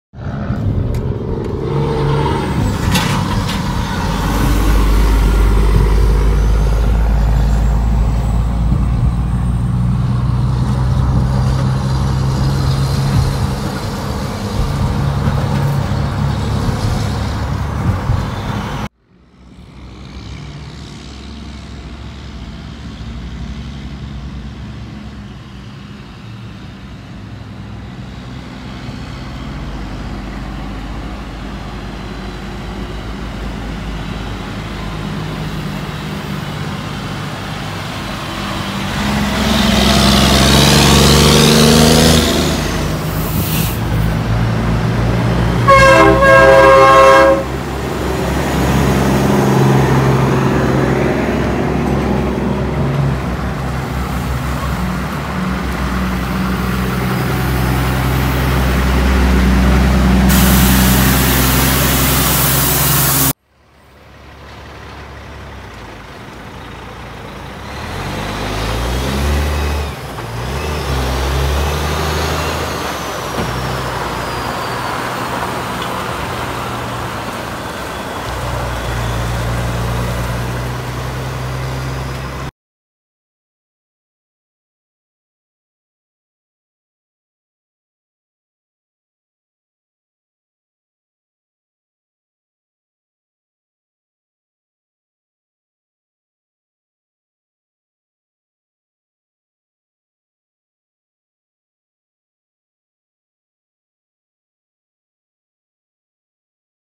Small Convoy - Ken Graham Trucking
A few Ken Graham trucks pulling into a parking lot.